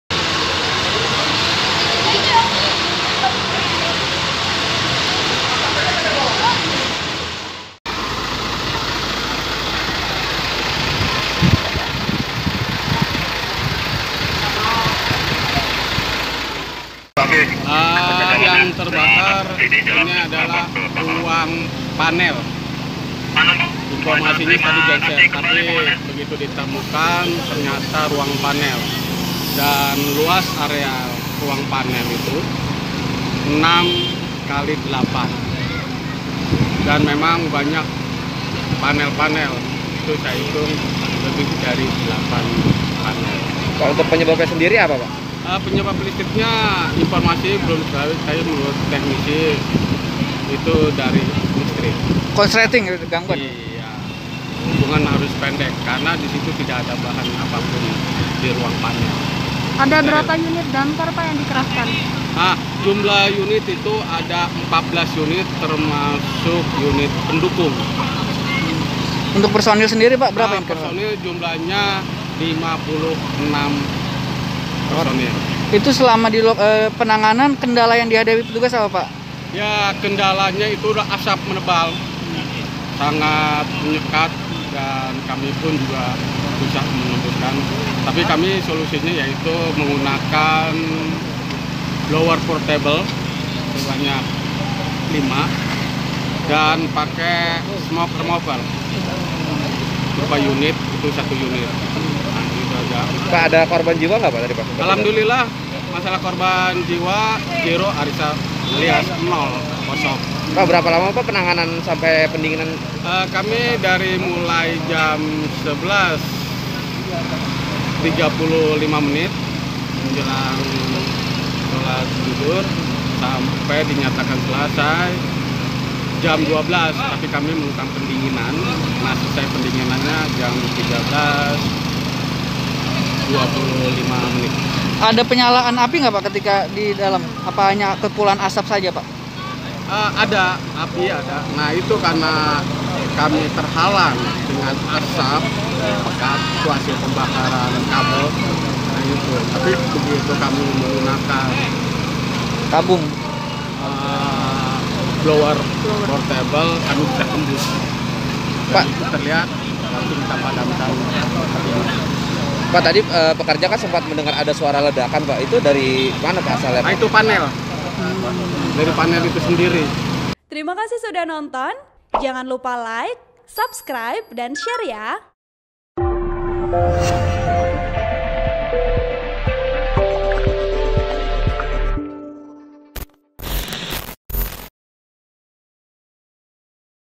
Kebakaran Melalap Lantai Basement Pasaraya Manggarai, Diduga Korsleting Listrik
Laporan Wartawan TribunJakarta.com, Satrio Sarwo Trengginas

TRIBUN-VIDEO.COM, TEBET - Kebakaran terjadi di lantai basement gedung Pasaraya Manggarai, Tebet, Jakarta Selatan.

Pihak Suku Dinas Gulkarmat Jakarta Selatan menerima berita sekira pukul 11.45 WIB pada Minggu (25/10/2020).

Perwira Piket Sudin Gulkarmat Jakarta Selatan, Sartono, belum bisa memastikan kebakaran.

Namun, menurut teknisi gedung, diduga korsleting listrik di ruang panel.

"Yang terbakar itu ruang panel. Informasi awal tadi genset tapi begitu ditemukan ternyata ruang panel," ujar Sartono saat ditemui di lokasi.

Luas area yang terbakar 6 x 8 meter.

Petugas damkar yang dikerahkan sebanyak 56 personel dan 14 unit pemadam kebakaran.

Asap tebal menjadi kendala utama petugas dalam menangani kebakaran itu.

"Asap tebal menjadi kendala sehingga kami sulit menembus titik kebakaran. Kita menggunakan blower portible dan smoke removal. Begitu asapnya hilang apinya kita padamkan," lanjutnya.

Sekira pukul 13.25 WIB, penanganan dinyatakan selesai dan dilakukan pendinginan.

Tidak ada korban jiwa dalam peristiwa tersebut.